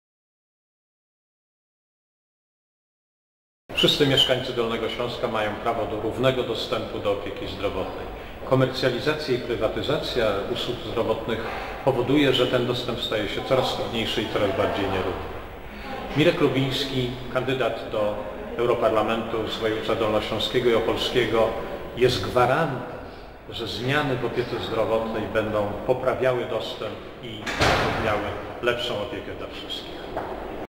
Mirosław Lubiński - kandydat Sojuszu Lewicy Demokratycznej do Parlamentu Europejskiego
Pozycja nr 4 na liście KKW SLD-UP.
Okręg wyborczy nr 12 (województwo dolnośląskie i opolskie).